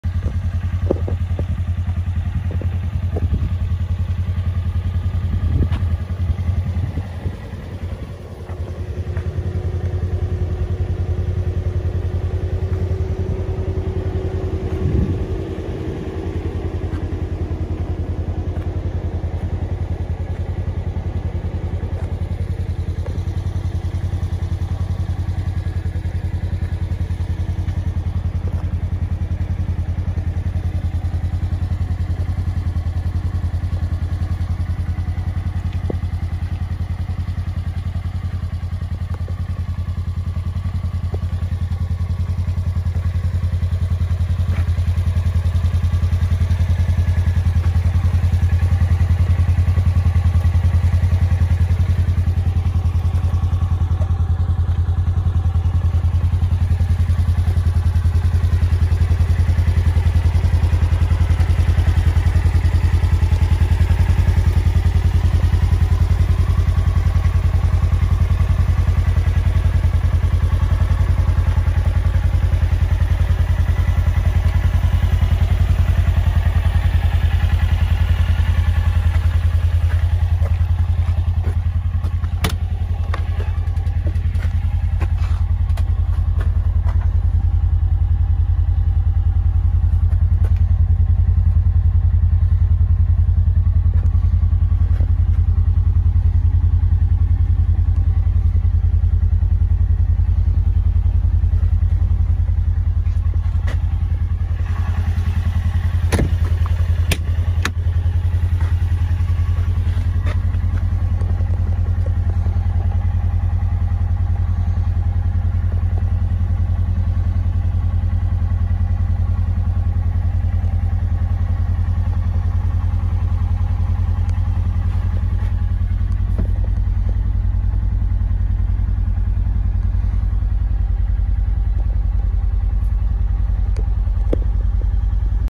2022 Polaris ranger XP 1000 NorthStar crew cab with audioformz roof.
custom built 2022 Polaris ranger xp 1000  NorthStar ultimate crew. Added 10 utv stereo
UTVS-654 mids in the roof & lower dash with led rings. Installed 2 utv stereo UTVS1000.5D amps. For bass we went with the 2 KICKER COMP RT 10" SUBWOOFERS in custom made 1cf enclosures rhino lines inside & out. There is a total of 2800 watts which is more than ample for me. Installed are the Chupacabra quero race mirrors. As you can see the HCR suspension with illusion purple powder coat. Went with the Thumper Fab Bumpers - Wheels are Metal FX delta contrast with 32x10x15 Xcomp tires - Gilomen ECU tune & clutch - Zbroz exit shocks - windows were ceramic tinted with 35% - NOCO Genius GENPRO10X2, 2-Bank, 20A (10A/Bank) Smart Marine Battery Charger with external plug in the rear driver side fender well. Dual utv stereo batteries
Assault Steering wheel with the assault adapter to attach the switch pros switch panel. Installed sound dampening throughout the NorthStar - Reverse camera was relocated to the rear of the tailgate As you can see - Turn signal kit by Xtc power. The wrap was done by 518 designs. Complete build was done by ( DJ DEACON  ) D&D Mobile Services.